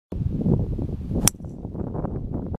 Bobby Clampett - 3 Wood Down the Line
Bobby Clampett from down the line, at Quail Lodge in Carmel, CA.